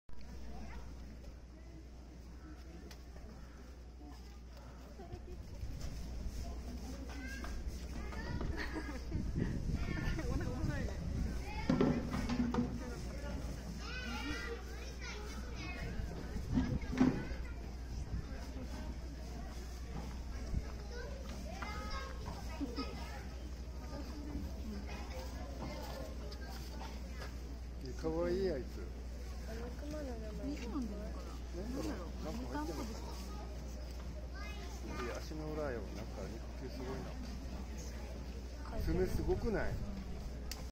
近夏★④マレーグマ・マレー熊・マーズくん・沖縄こどもの国・・・Malayan_sun_bear・Okinawa Zoo & Museum(20230403)
マレー熊＝まれーぐま＝マレーグマ＝Malayan_sun_bear

♂　オス　まあず=マーズ=Mars　２００３年４月２９日生
　　高知県立のいち動物公園
　　→　2004年5月11日 天王寺動物園
　　→　2021年2月15日（月）来沖

マレーグマ (馬来熊、Helarctos malayanus) は、哺乳綱食肉目クマ科マレーグマ属に分類される食肉類。本種のみでマレーグマ属を構成する。